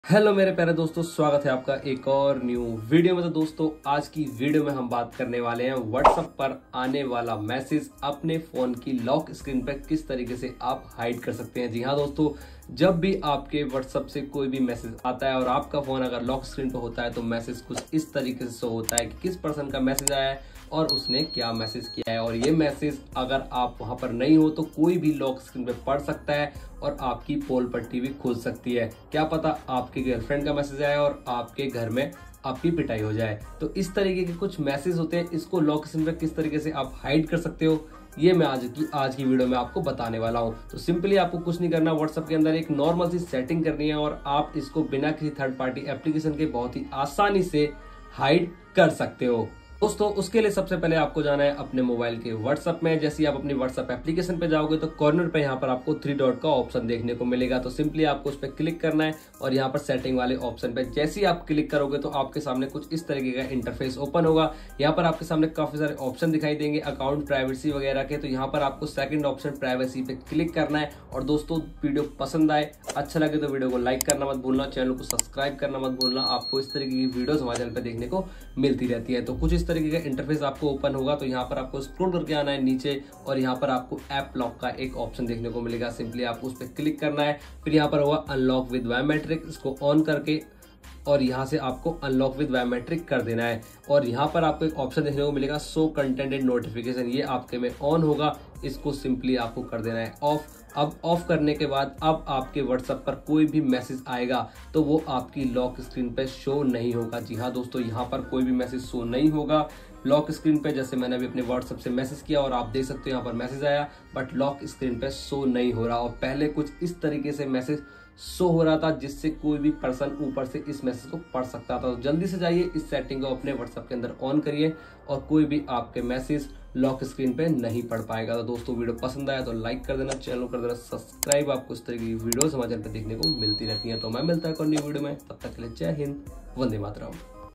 Whatsapp Notification Lock Screen पर कैसे Hide करे 2024
how to hide whatsapp notification content on lock screen